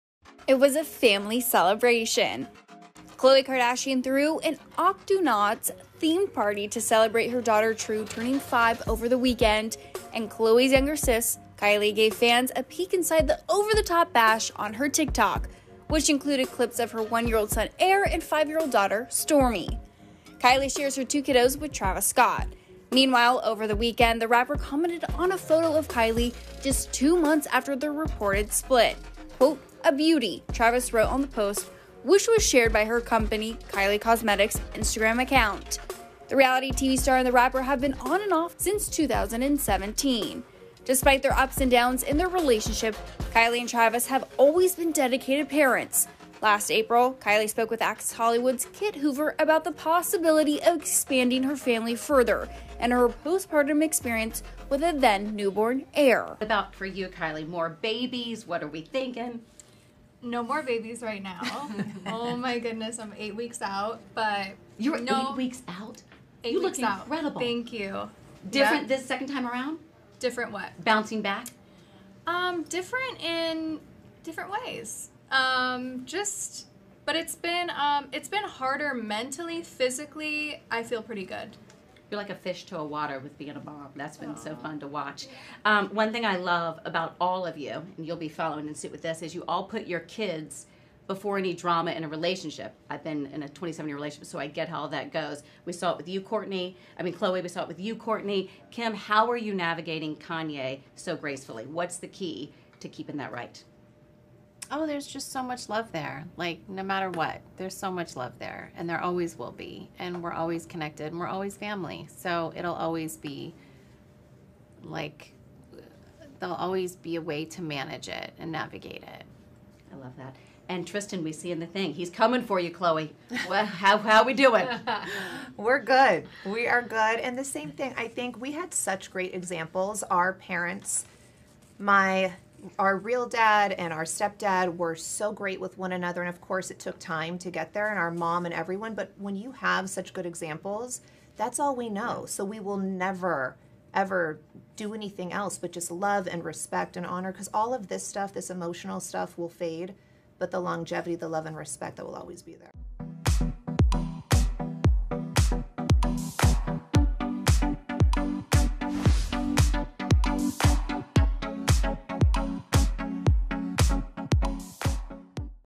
Kylie Jenner Shares RARE Clips Of Kids Aire & Stormi At Cousin True’s Bday Party
It was a family celebration! Khloé Kardashian threw an Octonauts-themed party to celebrate her daughter True turning 5 over the weekend. And Khloé's younger sis, Kylie, gave fans a peek inside the over-the-top bash on her TikTok, which included clips of her 1-year-old son Aire and 5-year-old daughter Stormi.